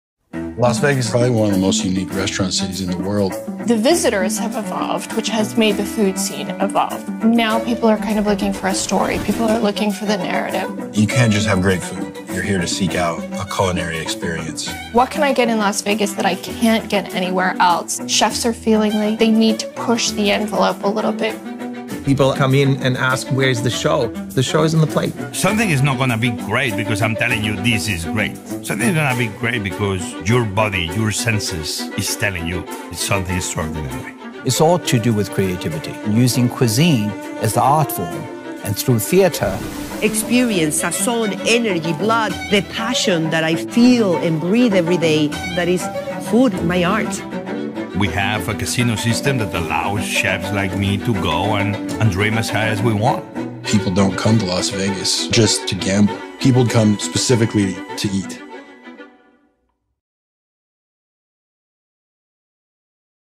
Vegas!! So much to experience in this unique city. Oh the food!! Major delectables await you!
Hear from top chefs what makes a meal worthy of michelin stars. Learn about new exciting unique twists being constantly invented in order to stay on top of the Vegas food scene.